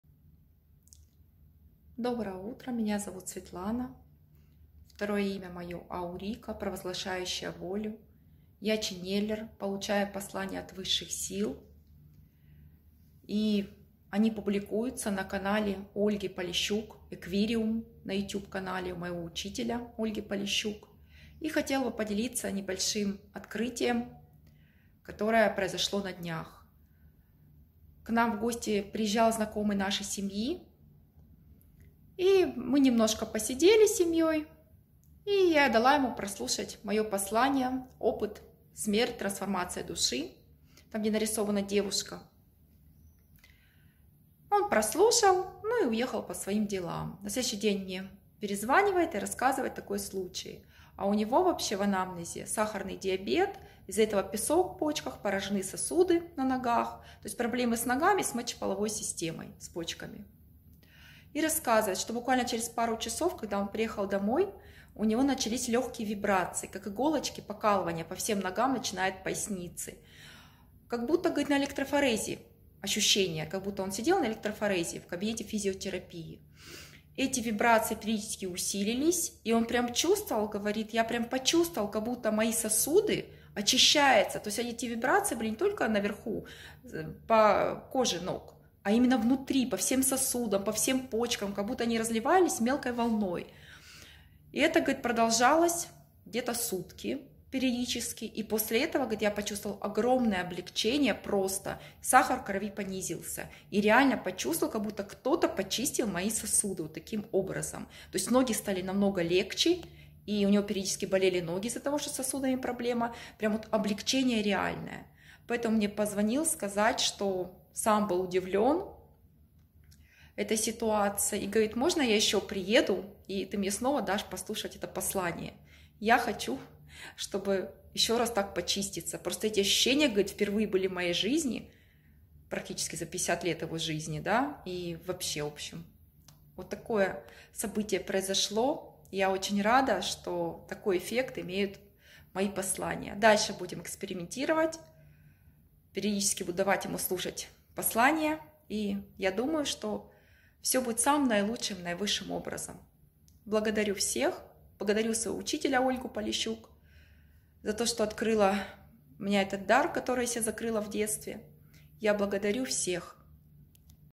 Отзыв о работе ченнелинга Аурика очищение вибрациями от сахарного диабета
Хочешь больше узнать про Род, родителей, Карму и способности, переходи по ссылке в мой канал